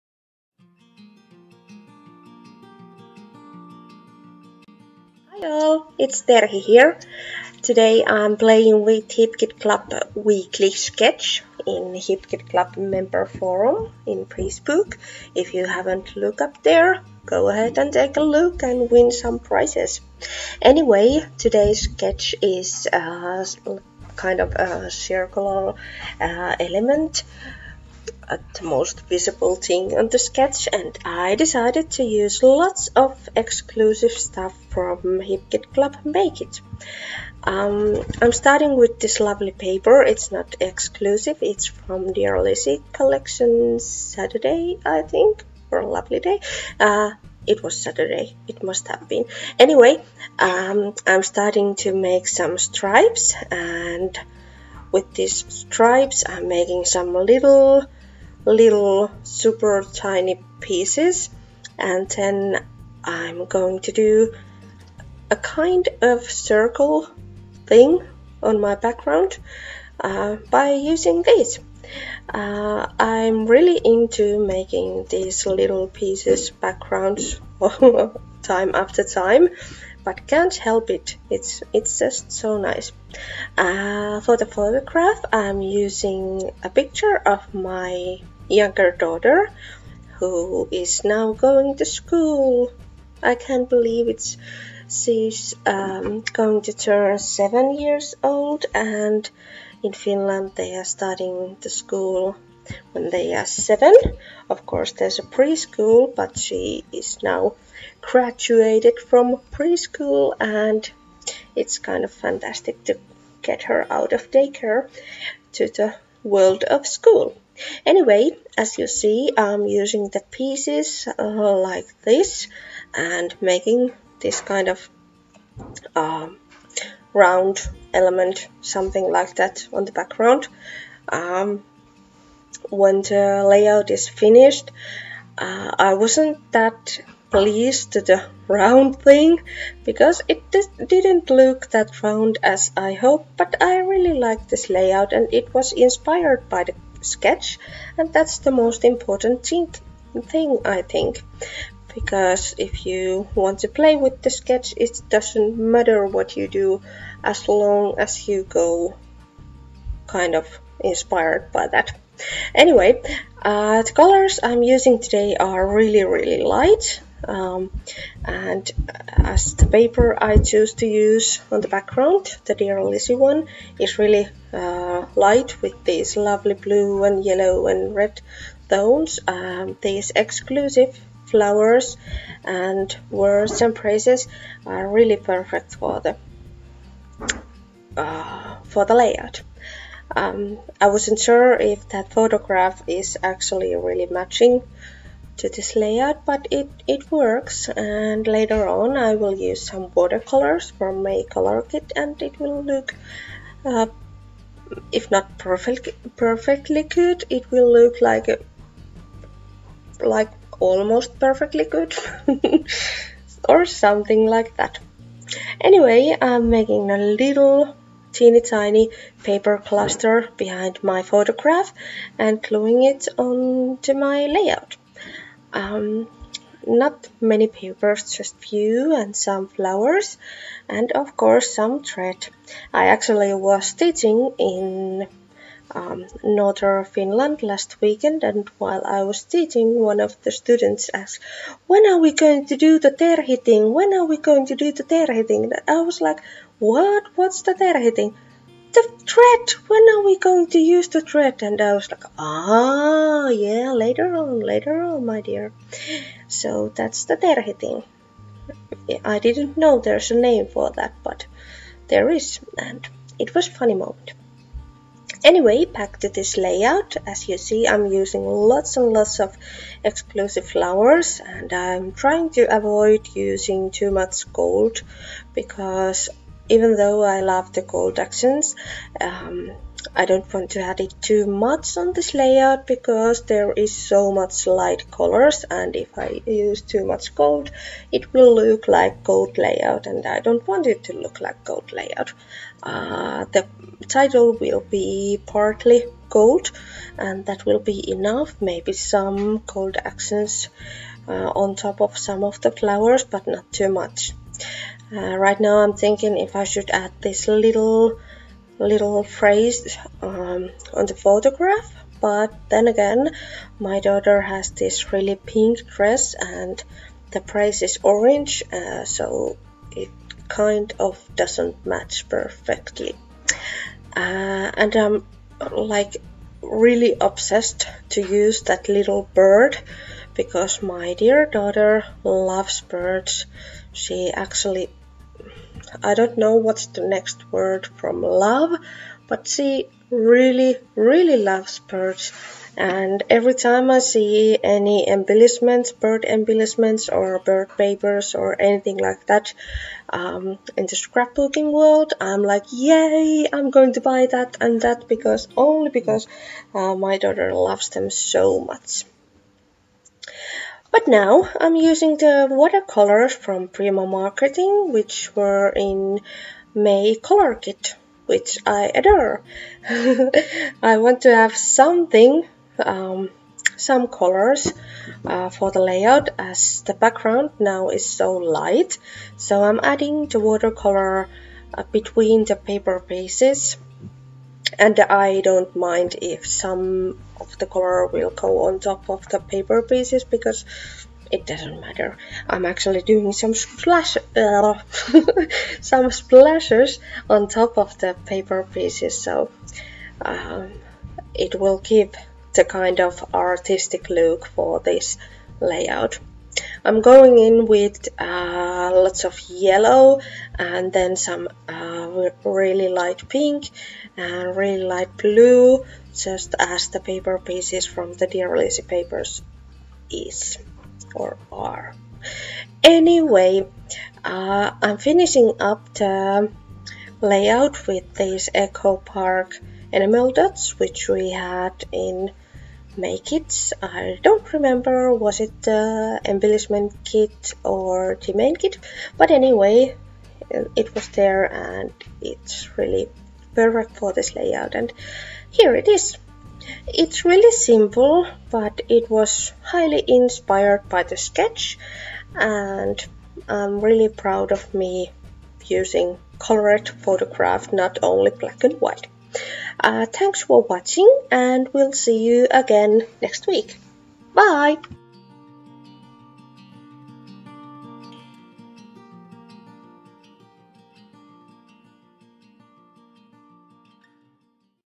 [196] Scrapbook layout for Hip Kit Club "Laughing Out Loud"
Scrapbook layout made for Hip Kit Club, using May kits. All available at ww.hipkitclub.net.